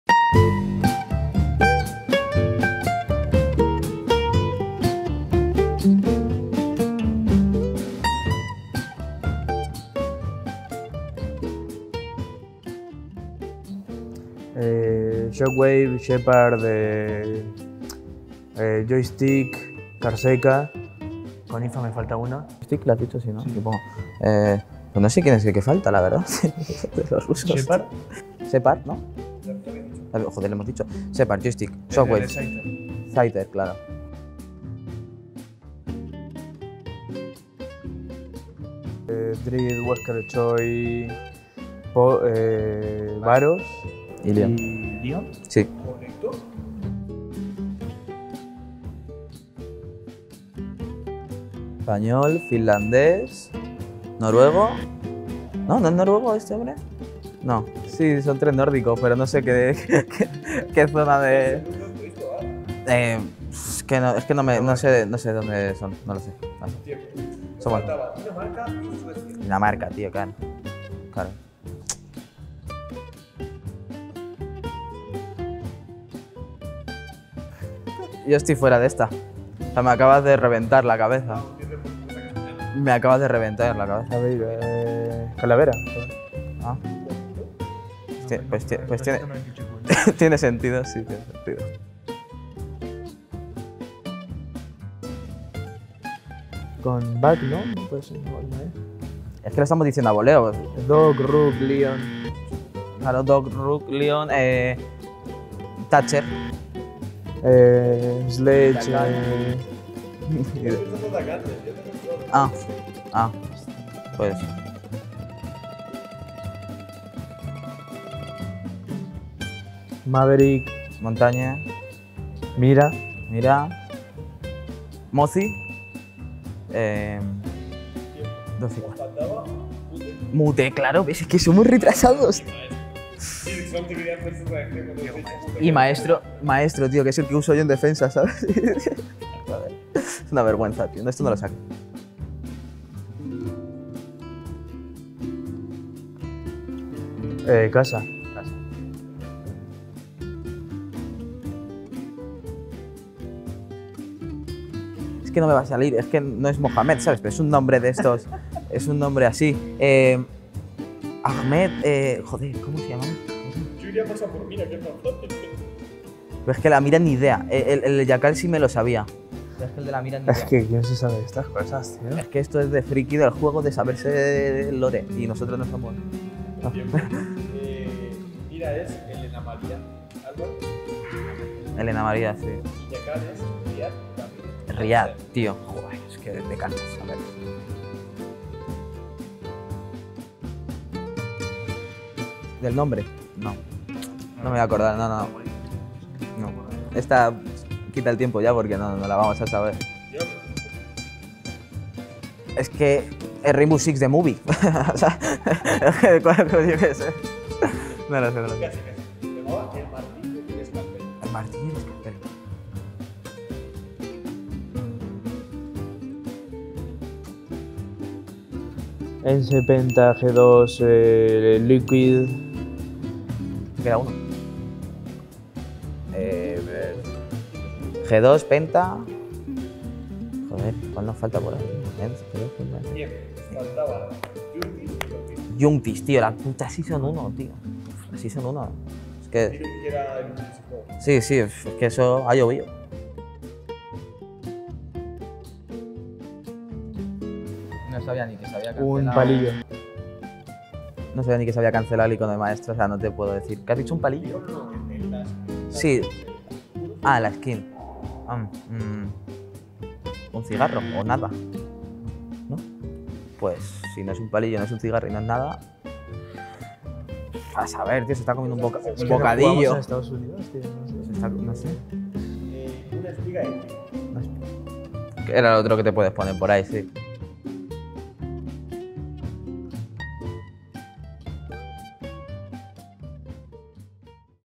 R6 Spain Nationals S1 Quiz - EFFICIENCY Team AntifaH y Sart3n
¿Cuanto saben los jugadores de EFFICIENCY Team sobre Rainbow 6 Siege?

Antifah y Sart3n responden a nuestro R6 Spain Nationals quiz.